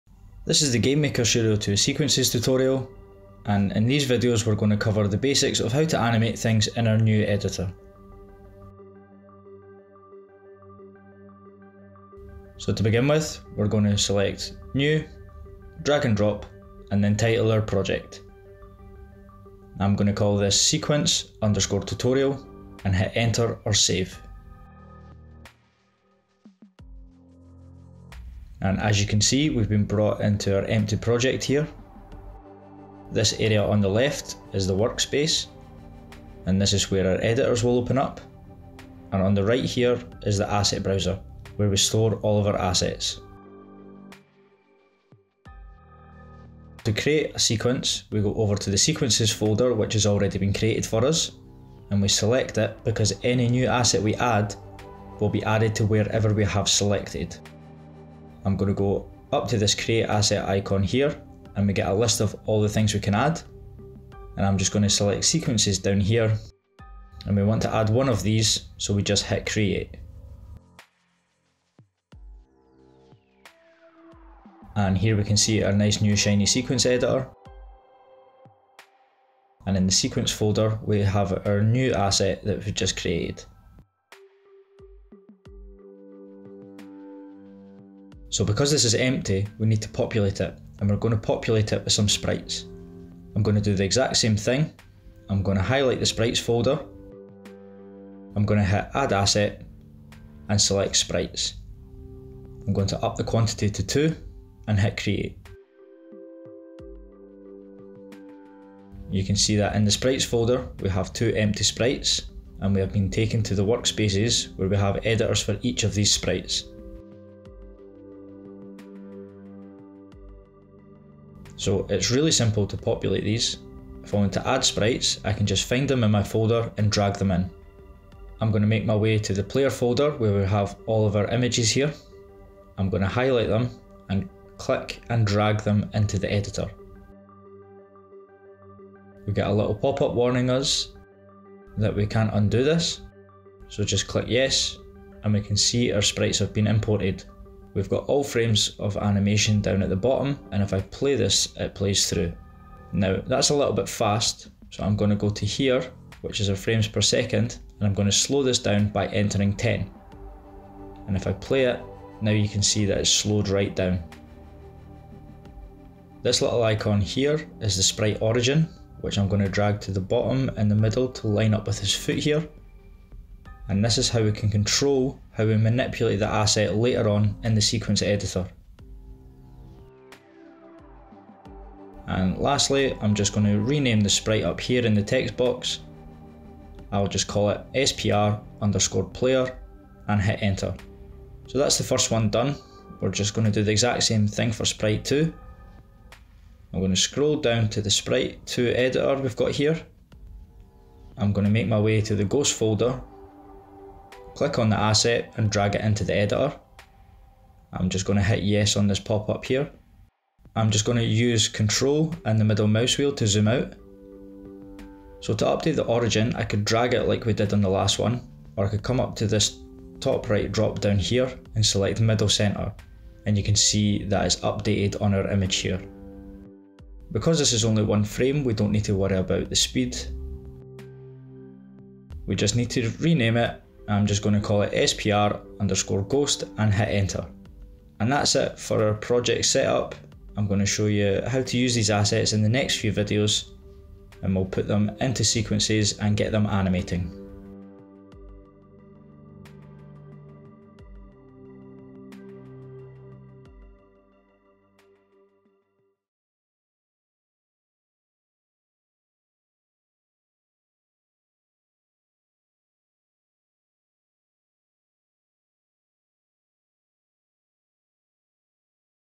Sequences Tutorial 1 - Project Setup
New in GameMaker Studio 2.3.0!

This tutorial covers the basics of setting up a new DnD project ready to use with Sequences.

Sequences helps developers and artists to easily create complex graphic effects in their games. These can include cutscenes and GUI-based manipulation of animations and audio, all of which can be positioned or context-triggered to occur anywhere within the game so anyone can achieve professional levels of polish to their games.

Topics covered:
Creating a project
Creating assets in the Asset Browser
Importing images to sprites
Changing the origin of a sprite